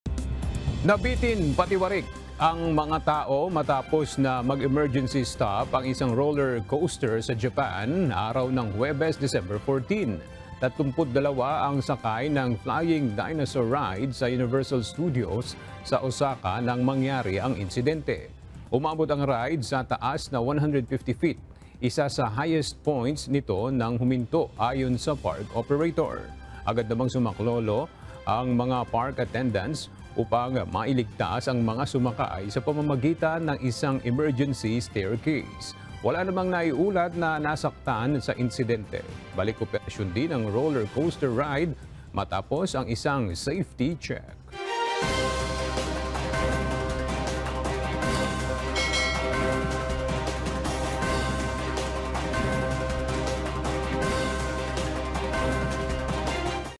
Roller coaster ride sa Japan, nag-emergency stop; mga sakay, nabitin nang patiwarik sa ere
Nabitin nang patiwarik sa ere ang mga sakay ng isang roller coaster ride sa Japan matapos itong mag-emergency stop nang dumating sa isa sa highest points nito na 150 feet.

Wala namang nasaktan sa insidente at balik-operasyon na rin ang ride matapos sumailalim sa safety check.